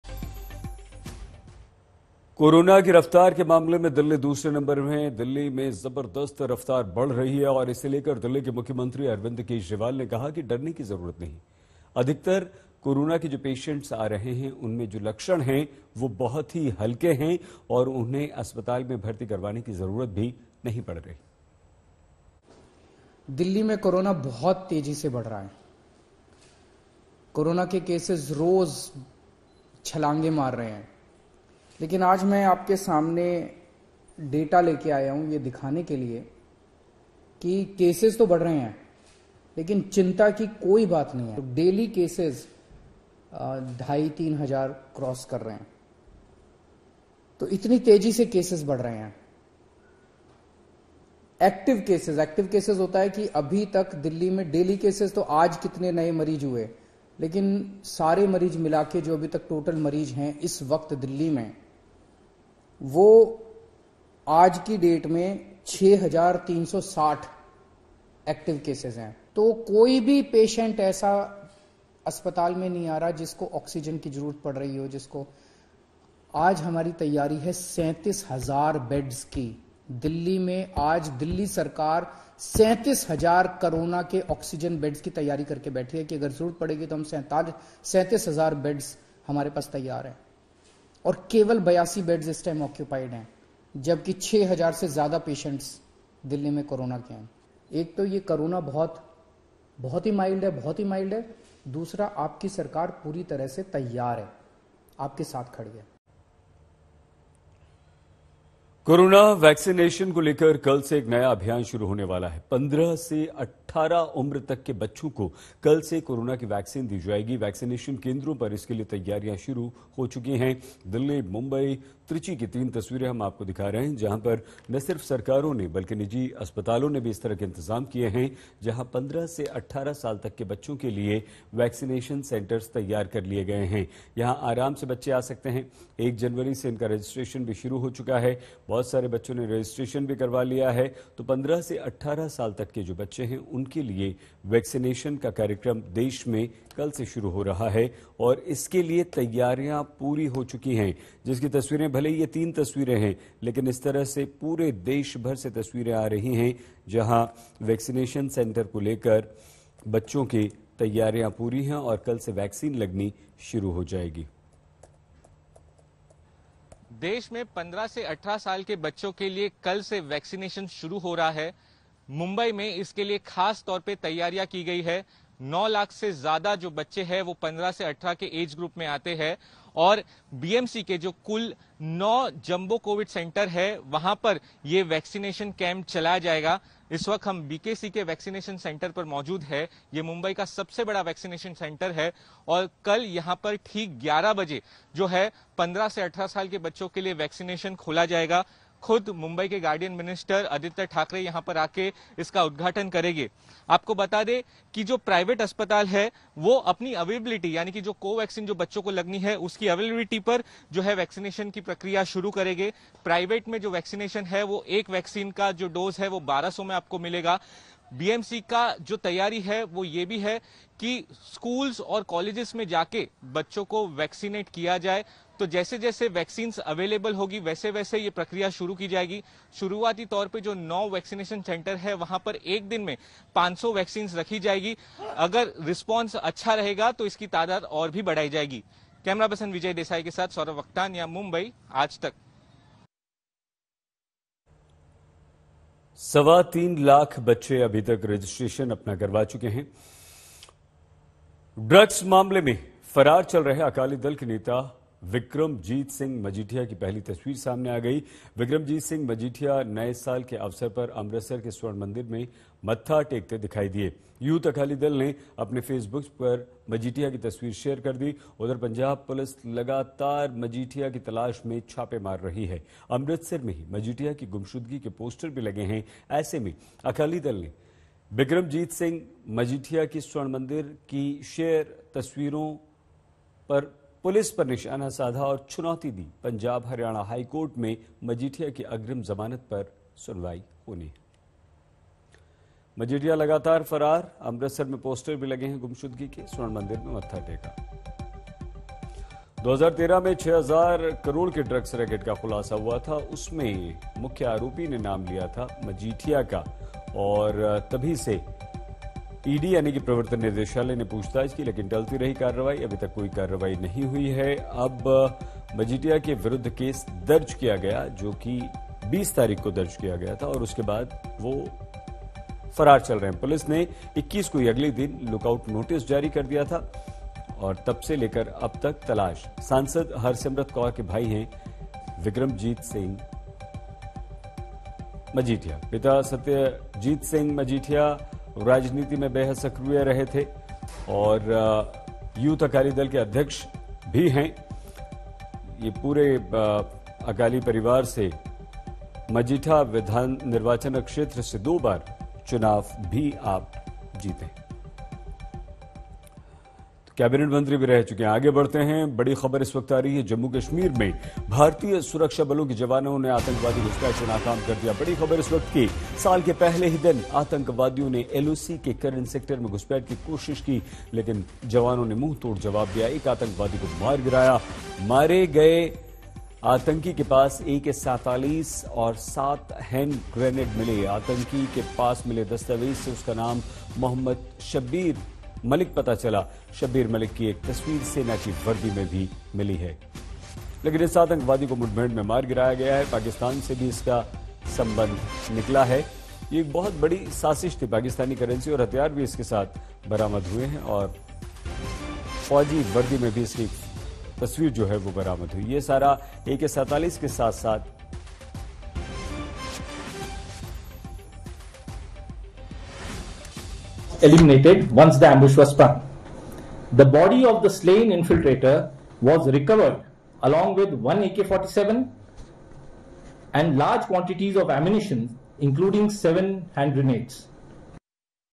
Corona पर Delhi CM Arvind Kejriwal का अलर्ट, कहा- 'तेज रफ्तार से दिल्ली में बढ़ रहा कोरोना' | Latest
Amid the surge in Covid-19 cases in the national capital, Delhi Chief Minister Arvind Kejriwal urged people to act responsibly, but asserted there was no need to panic as most infections are mild. Citing official data, Arvind Kejriwal said daily Covid infections in Delhi were growing by leaps and bounds with every passing day, whereas active cases have tripled in the last three days.